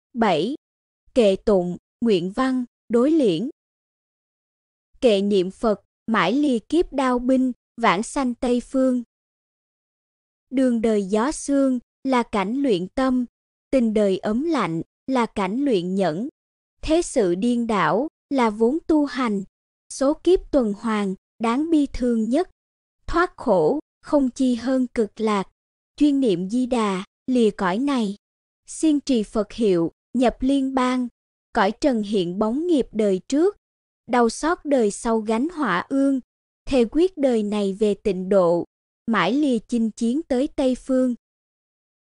158. Kệ niệm Phật mãi lìa kiếp đao binh, vãng sanh Tây Phương | Ấn Quang Pháp Sư Văn Sao Tam Biên Bổ
Ấn Quang Văn Sao Tam Biên Bổ
(phần bổ sung của Ấn Quang Văn Sao Tam Biên)
印光法師文鈔三編補
Chuyển ngữ: Bửu Quang Tự đệ tử Như Hòa
Giảo duyệt: Đức Phong, Huệ Trang và Chúc Bảo